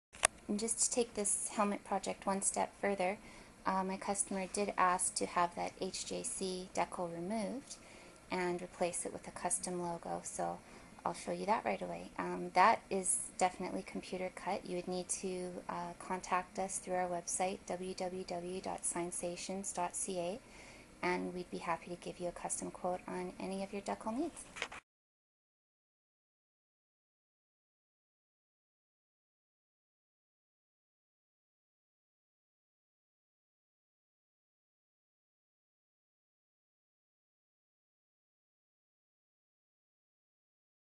Custom Helmet Part 2
small details make all the difference. See the difference when the commercial label is removed and replaced with the customers choice lettering.  Black Helmet decorated with Gold Reflective Vinyl film.